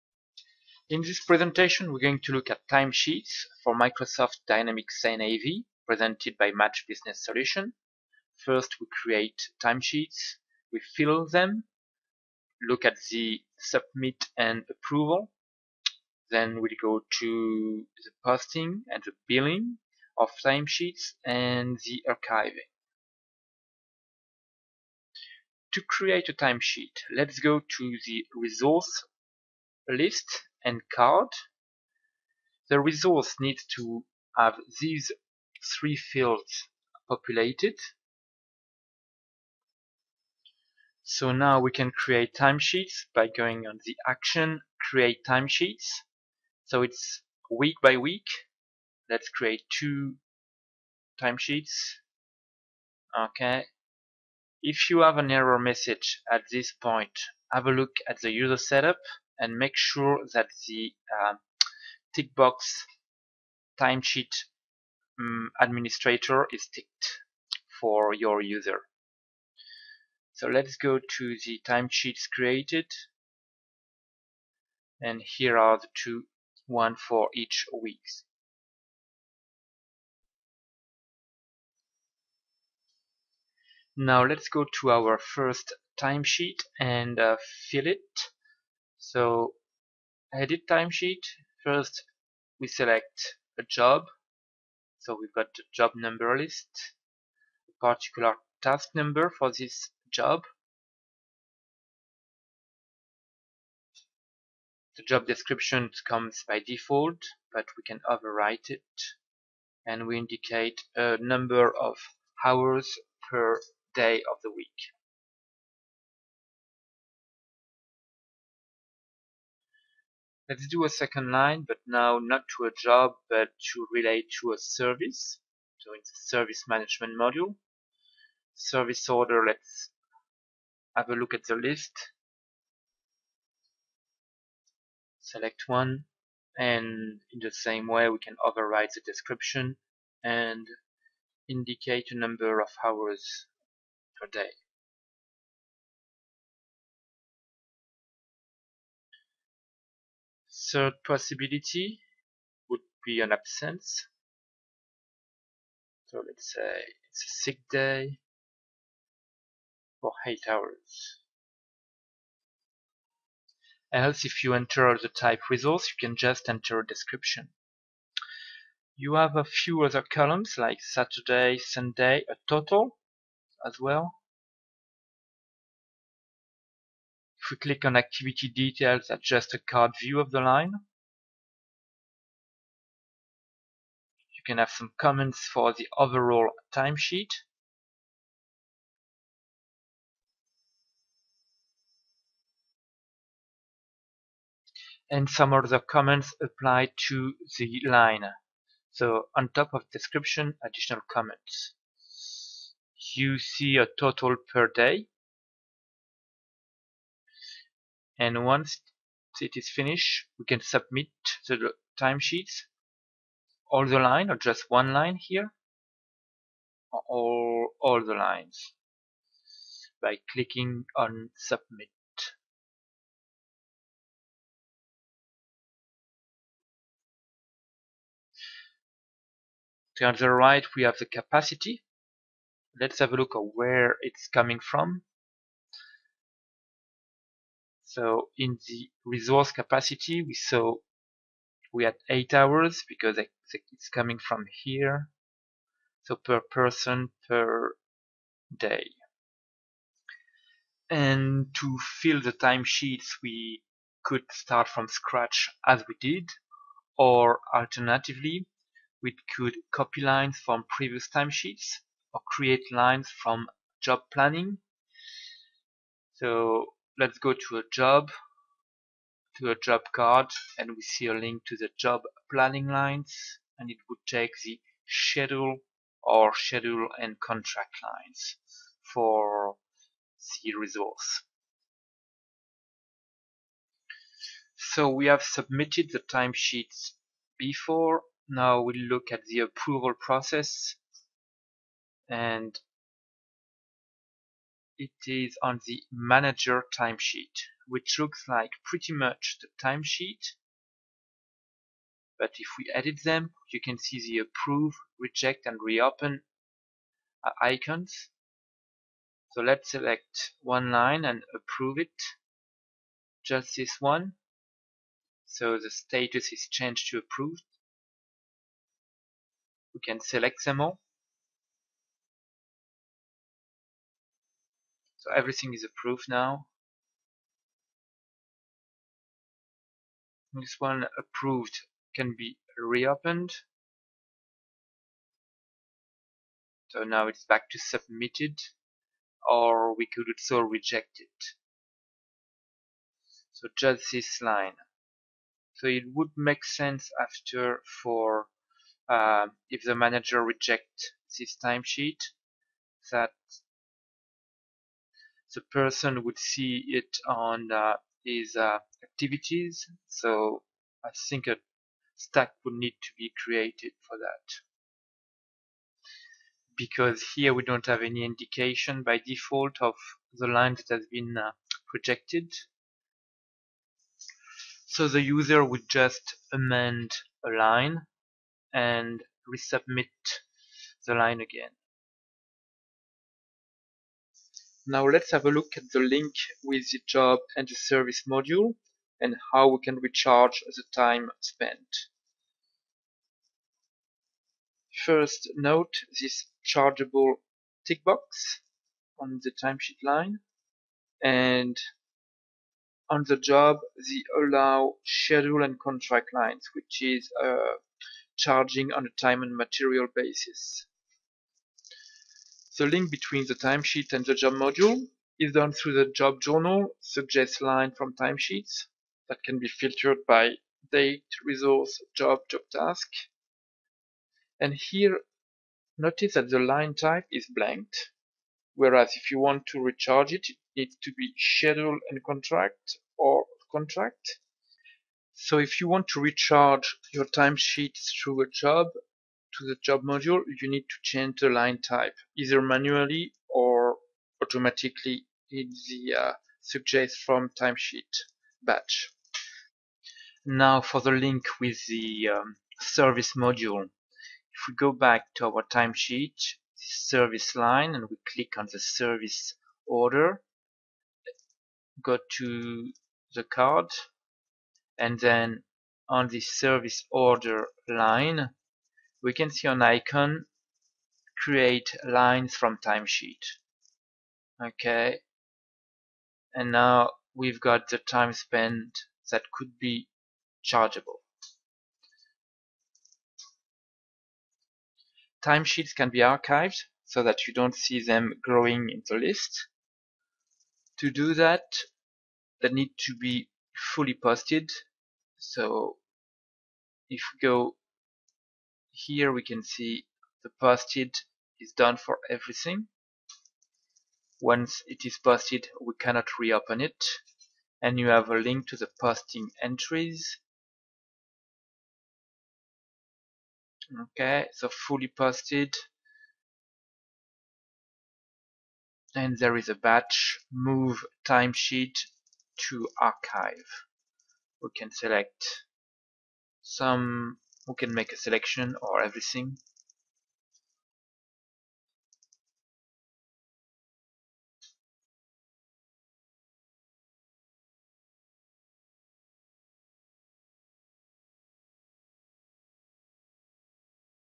How to use Dynamics NAV Timesheets
How to use Dynamics NAV Timesheets, presented by Match Business Solutions